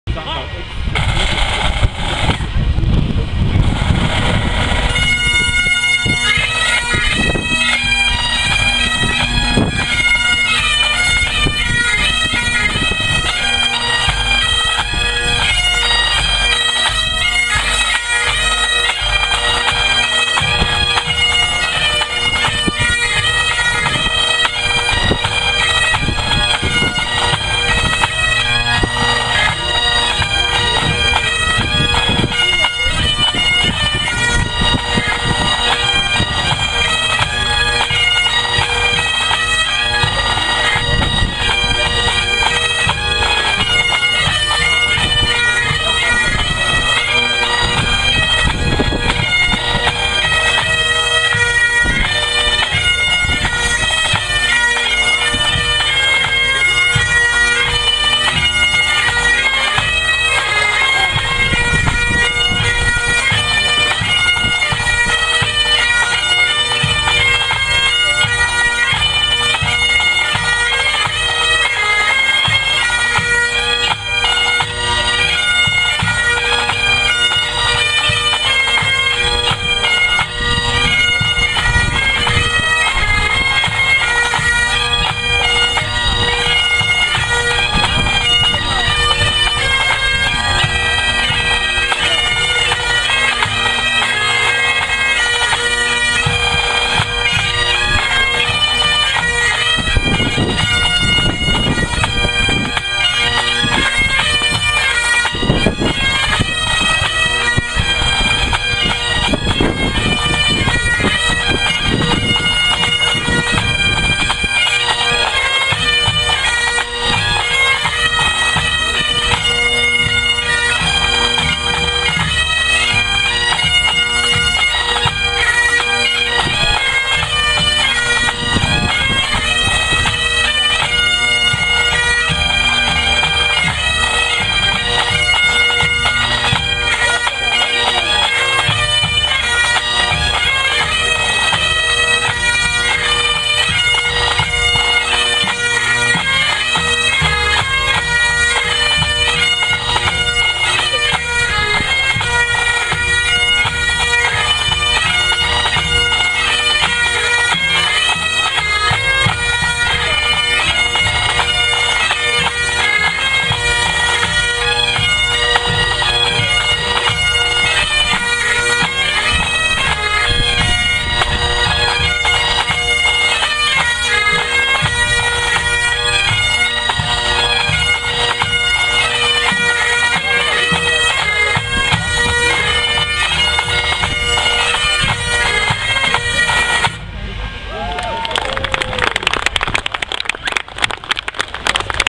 Bowhill and District Pipe Band
Bowhill and District Pipe Band play the grade 4 branch competition and win 1st place at Markinch Highland Games on 7 June 2015